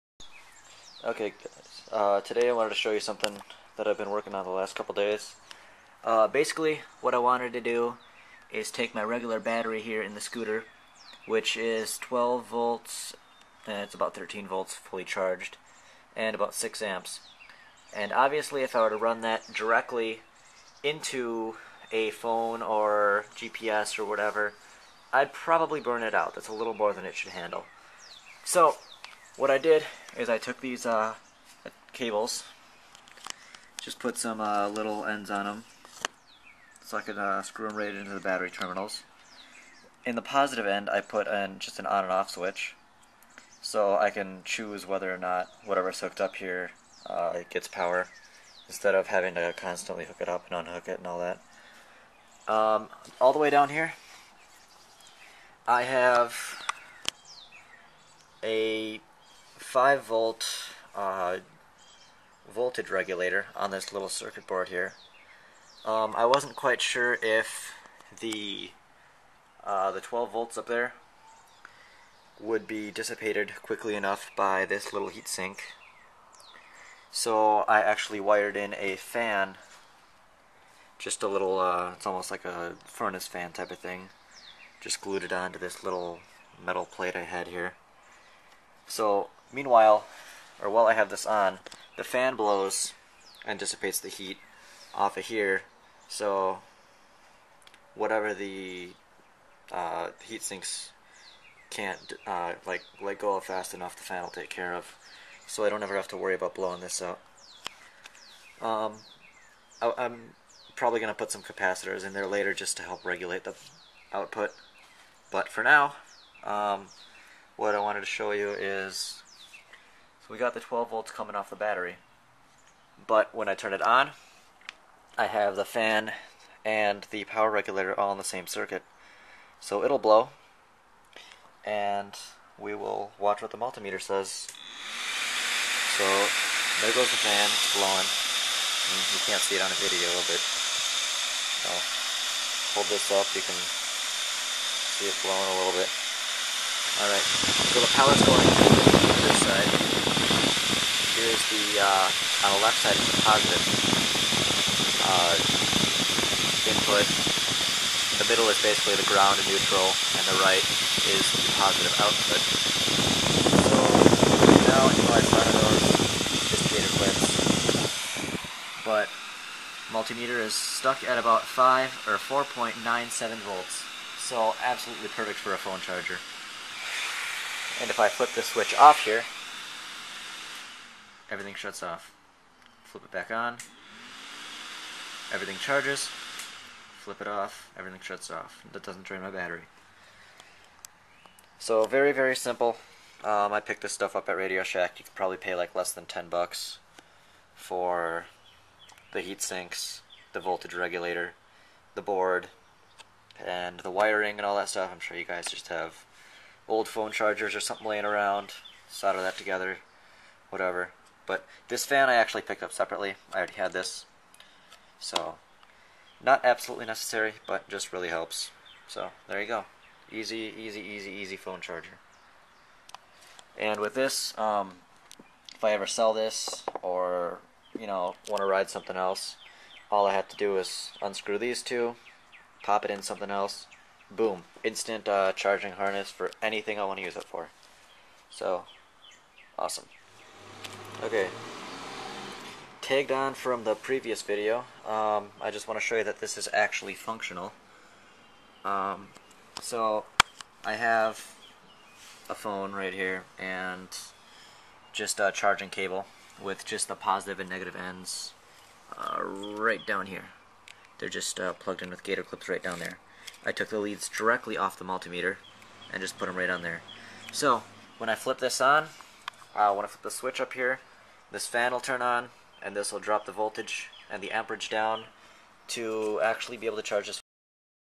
Car battery to phone charger
How to wire a car battery to a phone charger. is the link to the demo and update. Unfortunately, that part of this video got cut off.